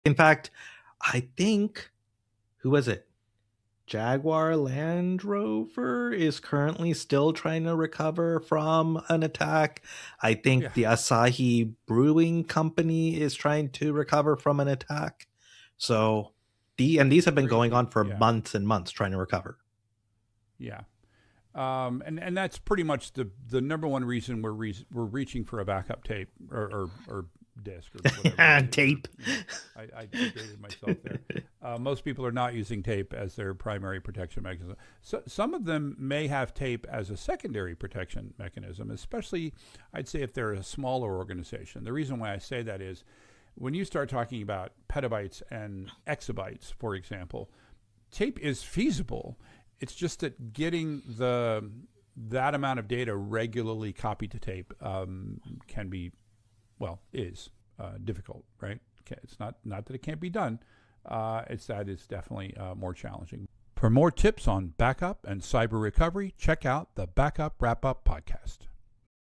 Ransomware Attacks: Why Backup Tapes Are Still Essential!
Jaguar Land Rover and Asahi Brewing still struggle after cyberattacks. Why backup tapes are making a comeback for data protection, even in the age of petabytes and exabytes. Is tape the answer?